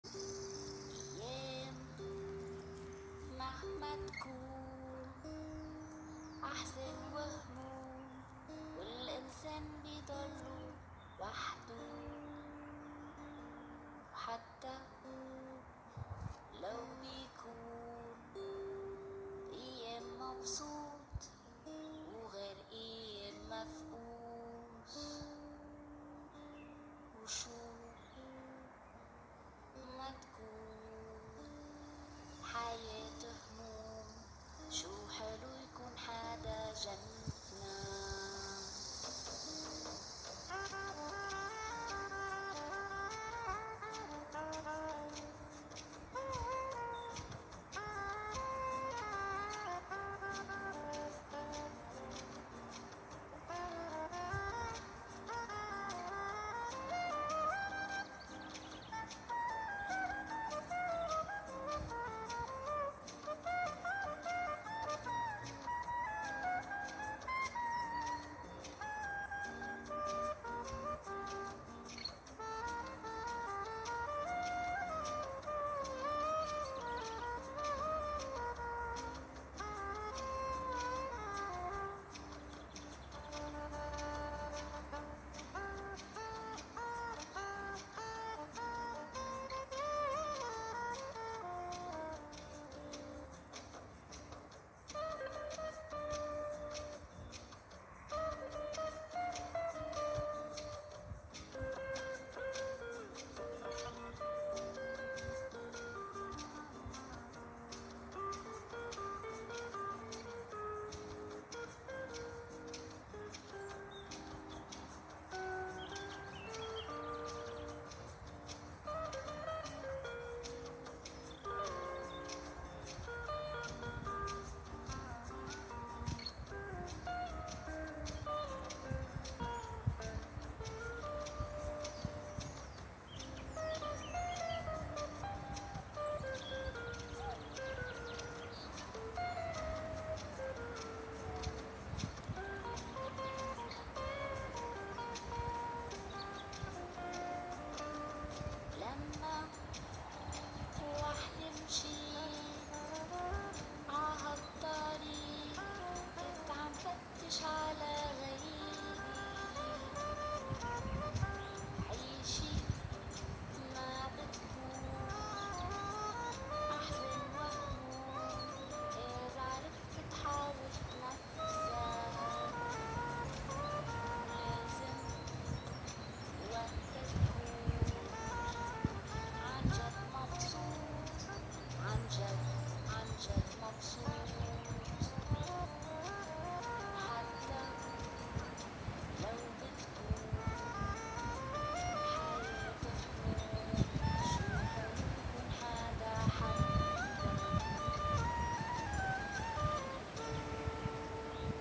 Impromptu Movement with spinal mobility and variations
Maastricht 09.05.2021
an unexpectedly warm day in Maastricht
spinal mobility from basic level of margaretmorrismovement
and some free improvisation on the variations, with the beautiful jazz music with arabic lyrics
I love this music by Salma Nova